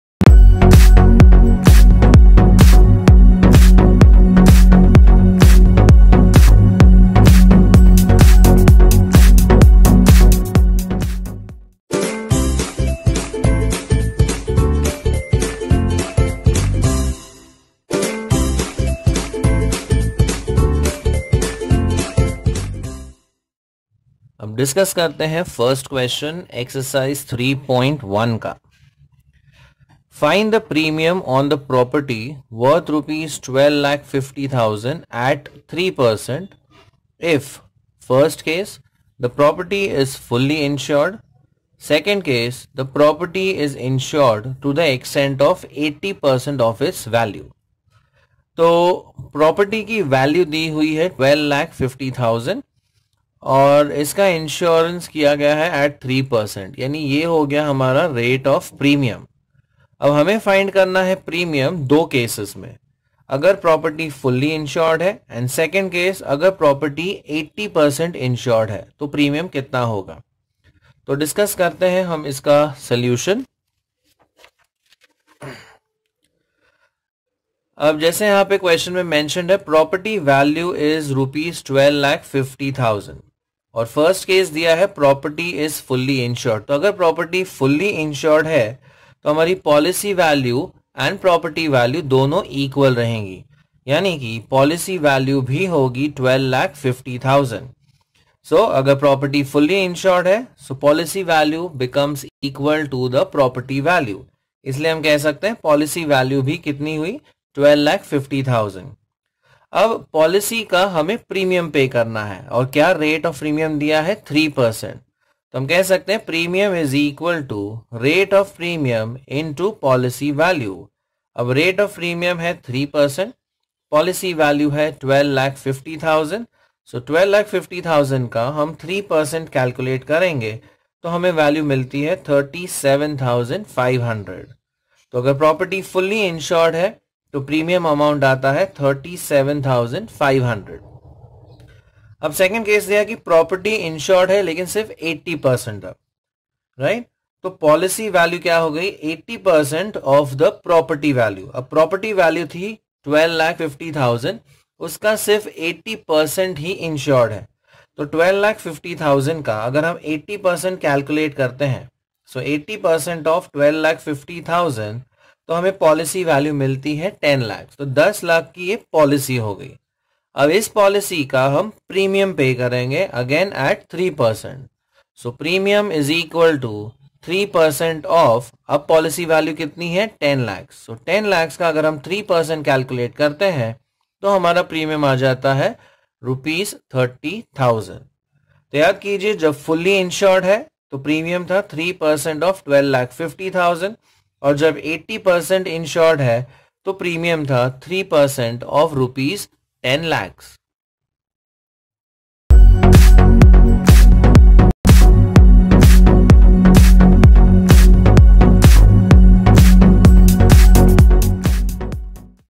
Insurance and Annuity - HSC - SYJC - 12th COMMERCE - Chapter 3 - Maths 2
Solution to Ex. 3.1 Problem 1

Find the premium on property worth Rs. 12,50,0000 at 3% if (i) the property is fully insured (ii) the property is insured to the extent of 80%  of its value.